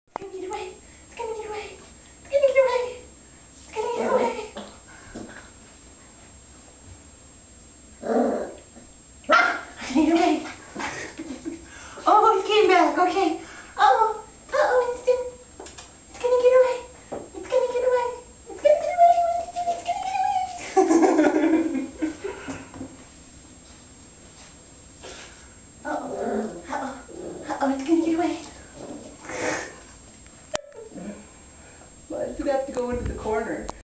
Welcome home, Winston!
Winston's first day home, playing in our puppy-safe bathroom. He's a red miniature poodle with a great disposition!